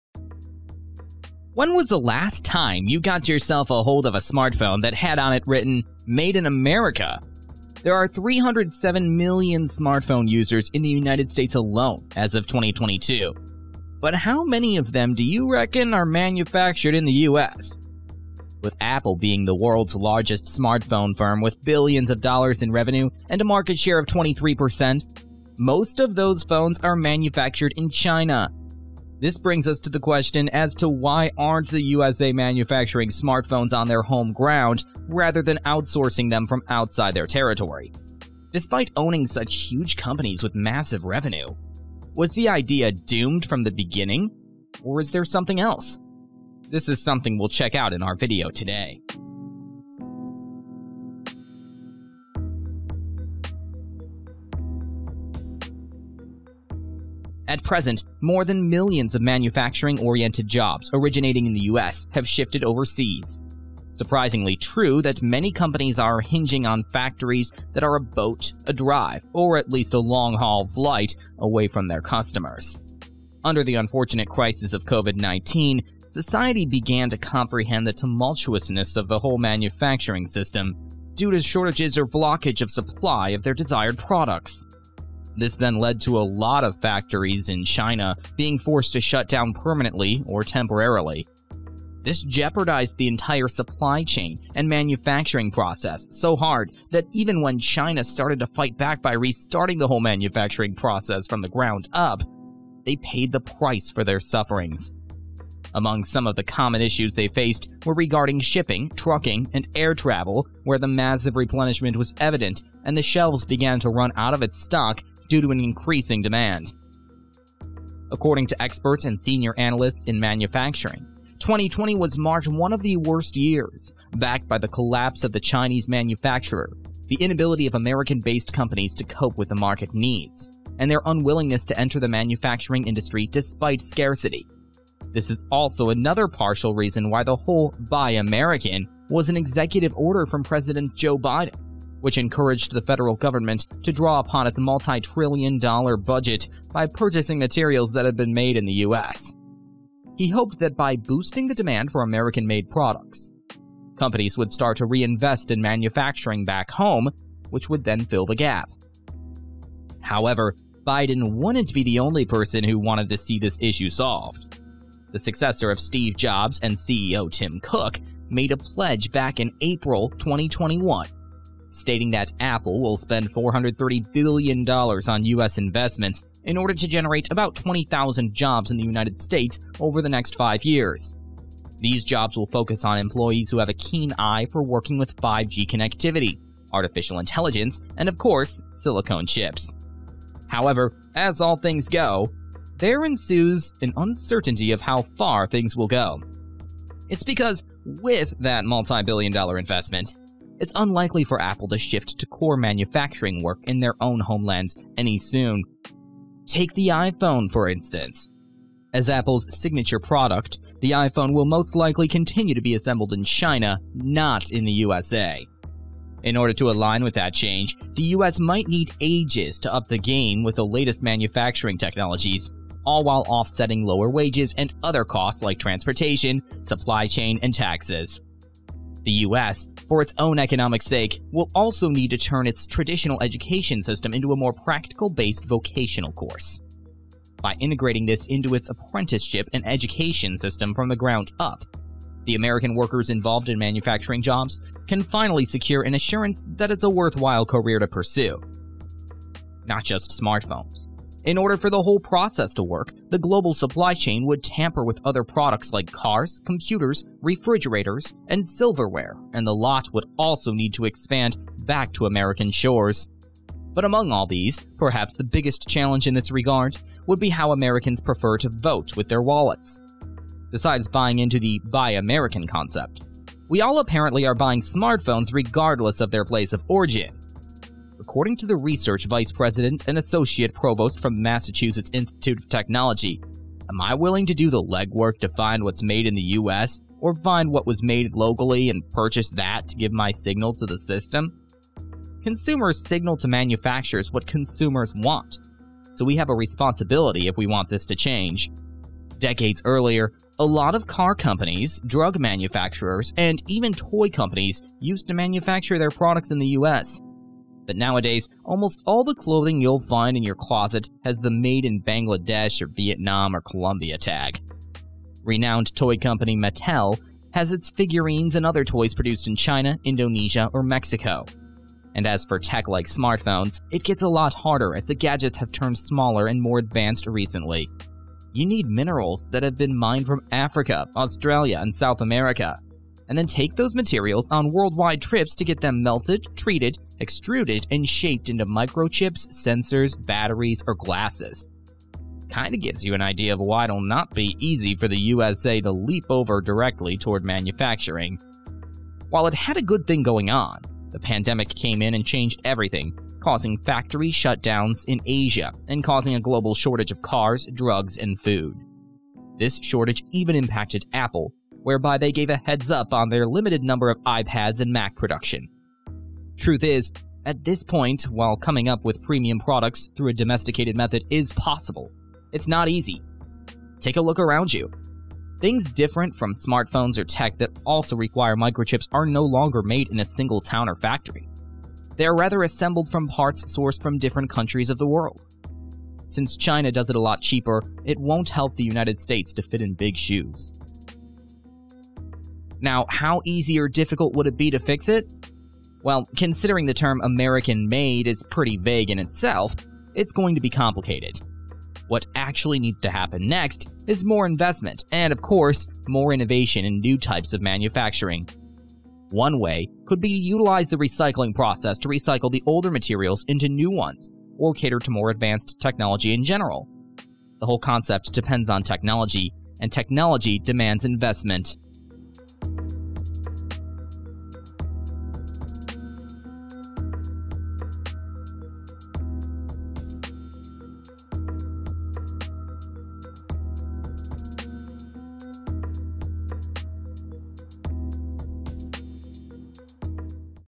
Why Your Phone is NOT Manufactured in The USA
While most cell phones you see are made in China, does it ever make you wonder why smartphones are not manufactured in the USA even if the company originated in America? That's what you will find out in our video today, as we share the details on smartphone manufacturing hassles, and also why your phone is not made in the USA